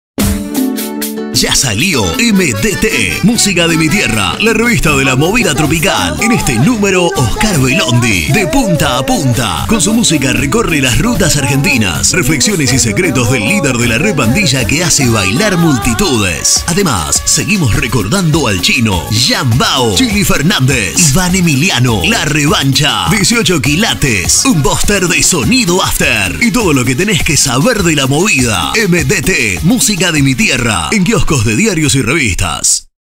SPOT MDT (MÚSICA DE MI TIERRA) -MARZO 2018
Spot de la edición de Marzo de 2018 de la revista Música de Mi Tierra.